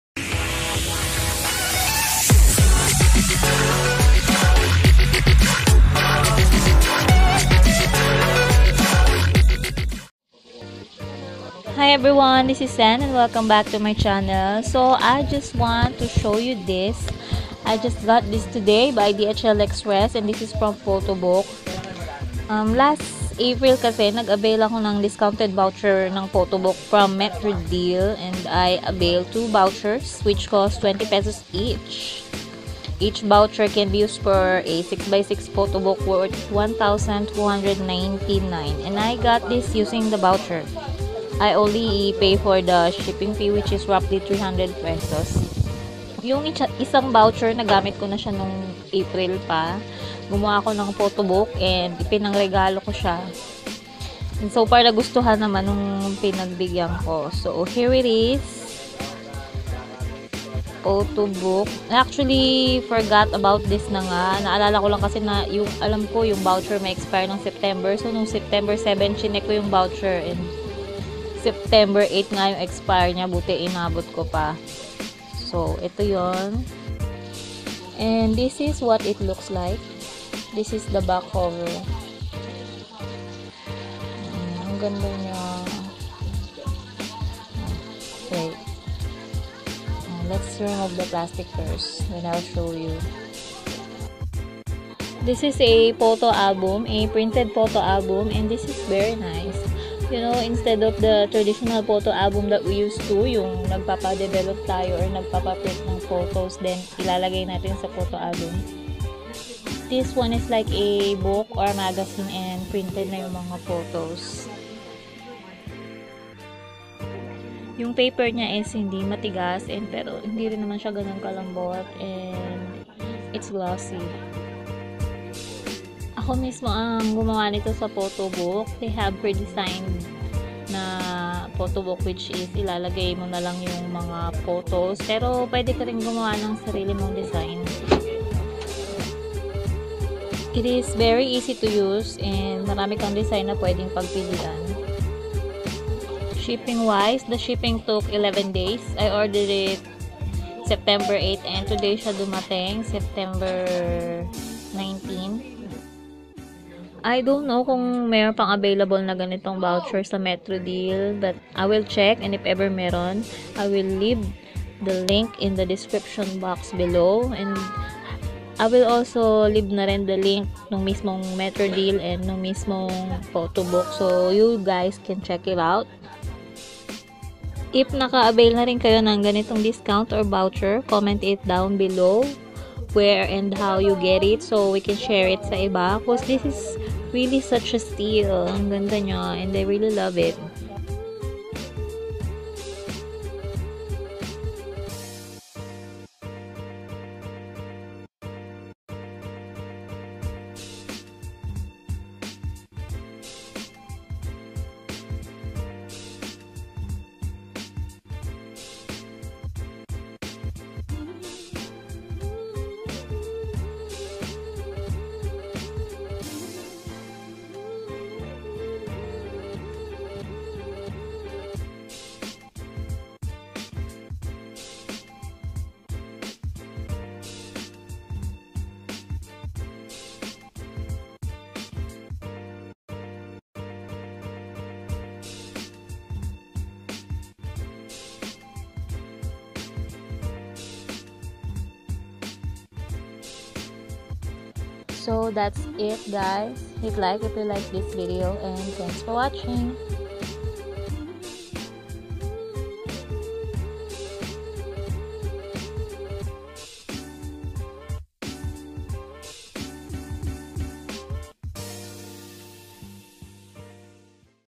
Photobook : using 20php voucher
I just want to share how i get this photobook for only 20php + shipping fee.